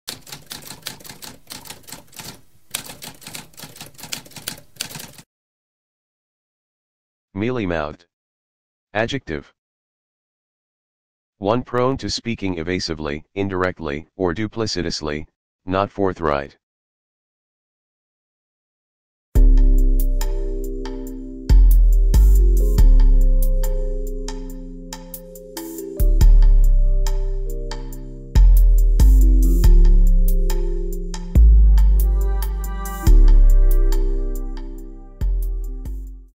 What does mealy-mouthed mean?
A spoken definition of mealy-mouthed.

Intro Sound:
Typewriter - Tamskp